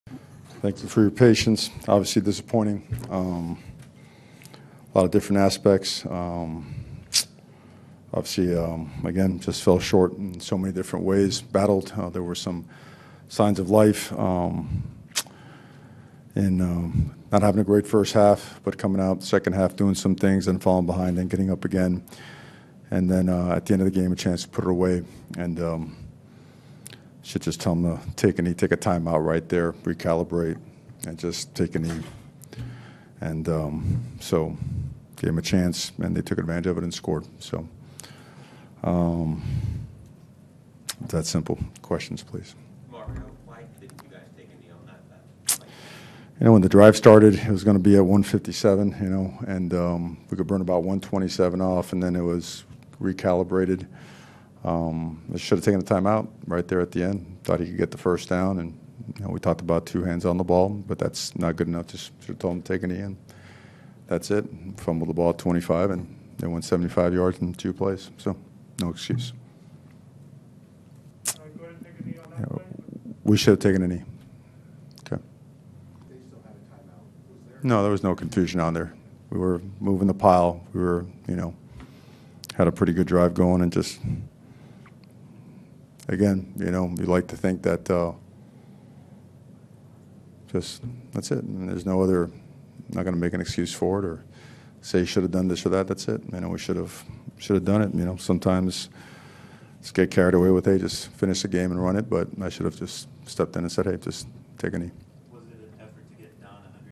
Mario Crisotbal Faces Music after Blunder Costs Miami Hurricanes vs. Georgia Tech
The Miami Hurricanes snatched defeat out of the jaws of victory in a 23-20 loss to Georgia Tech. Head coach Mario Cristobal faced the music after one of the most improbable losses in college football history.